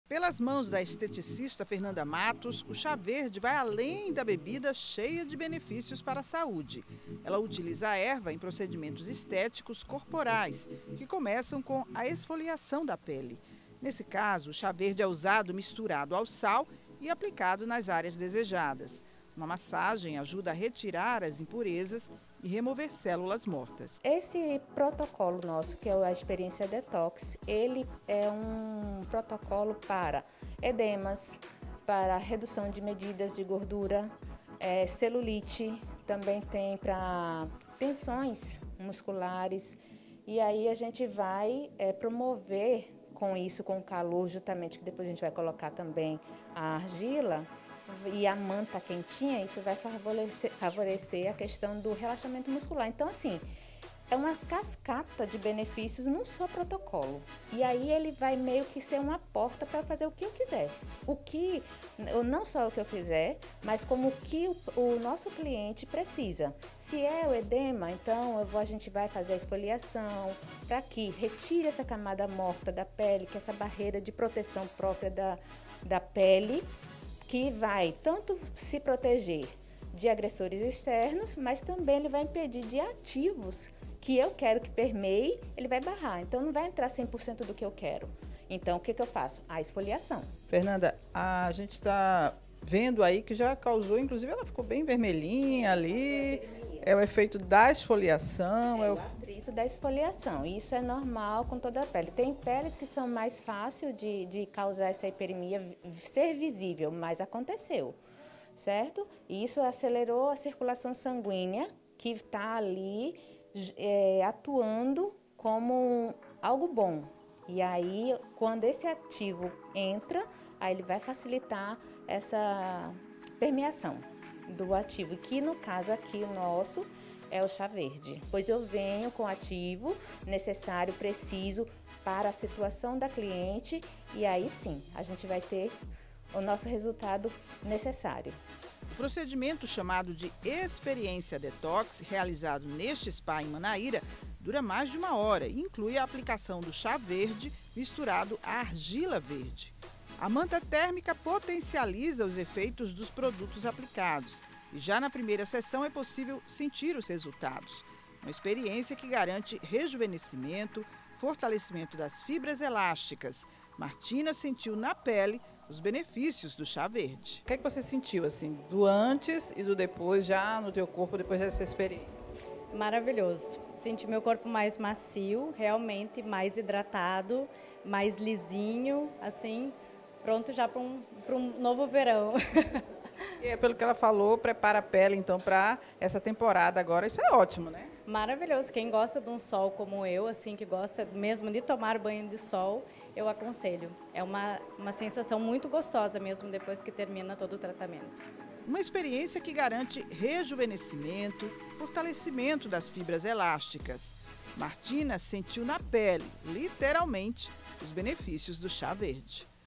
Chá verde promove na pele maior elasticidade e rejuvenescimento
Quando a gente ouve a palavra chá verde logo pensamos numa xícara bem quentinha, né? Só que os benefícios desse chá vão além da ingestão. Na estética ele pode ser aplicado para fazer um detox corporal e deixar a pele mais elástica e rejuvenescida.